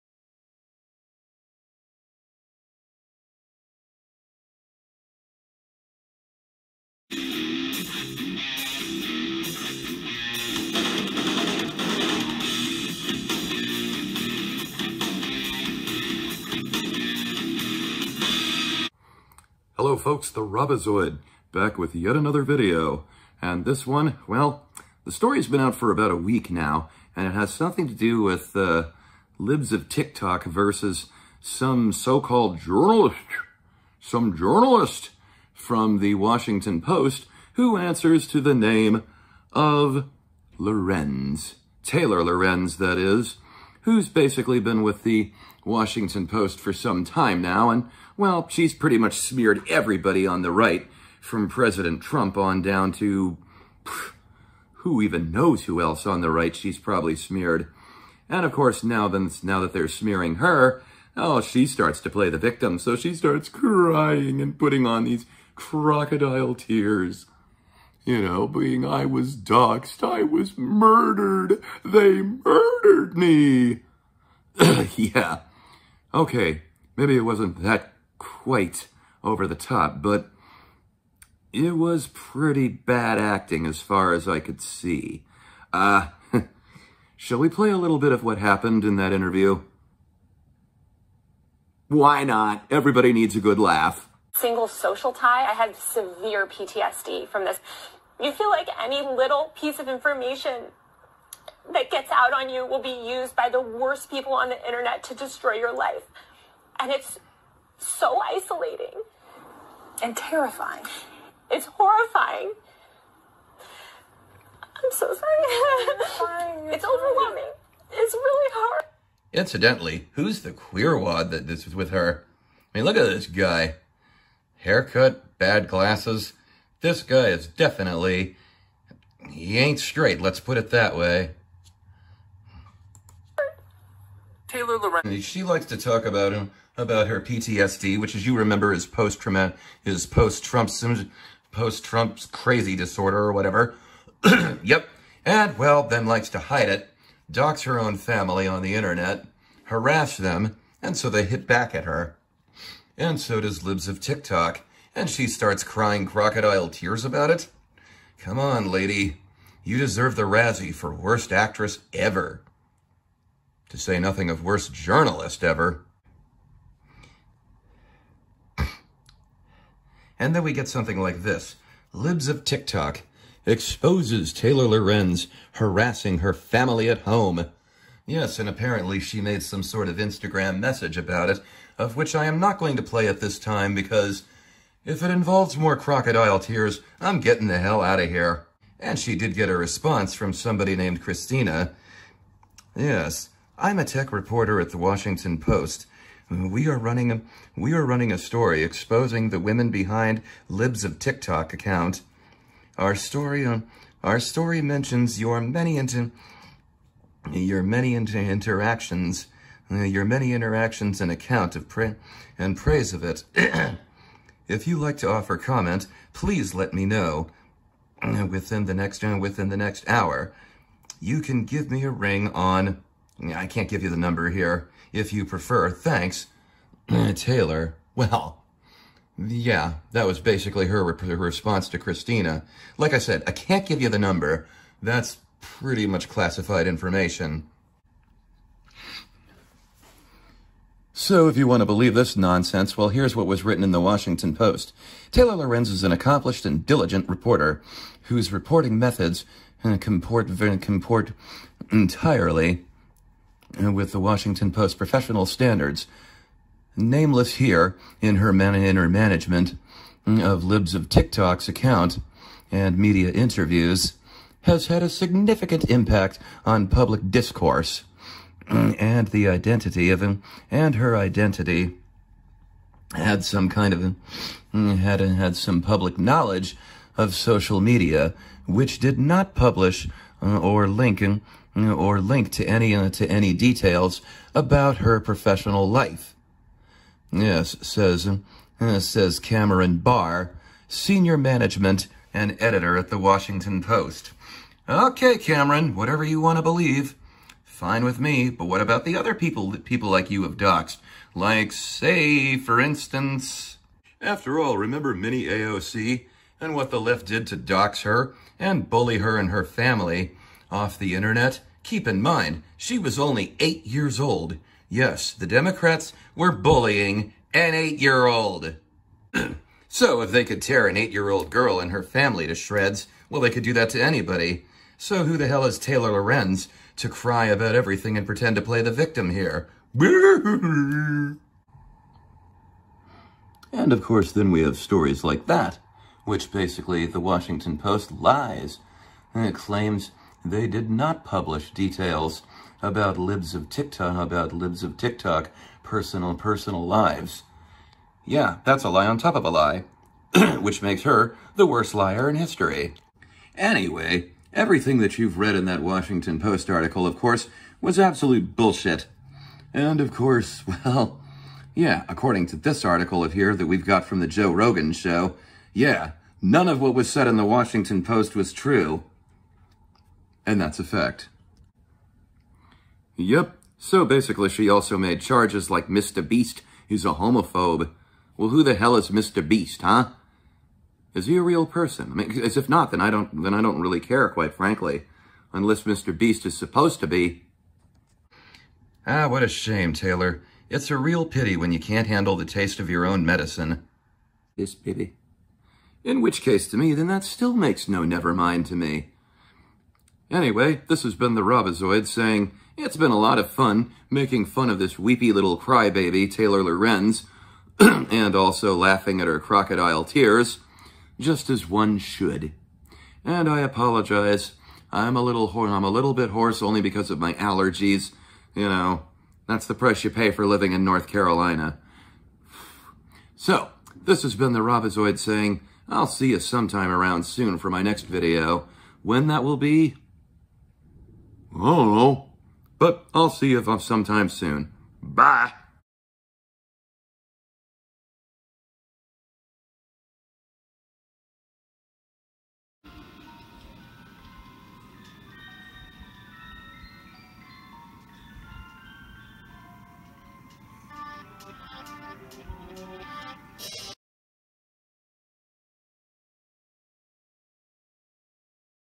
Taylor Lorenz's Crocodile Tears
You know this story like the back of your left hand by now. Washinton Post journalist (So-called) goes on MSNBC (More Stupid Nonsense By Communists) and cries fake tears about SHE"S the victim of doxxing!